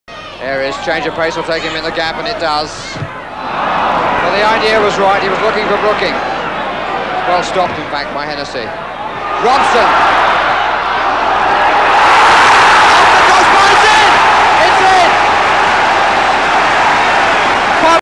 Brian 'Pop' Robson Goal - West Ham United v Derby County
Pop Robson scores a great acrobatic for West Ham in the 70's. I don't know who the team that he scored against are though.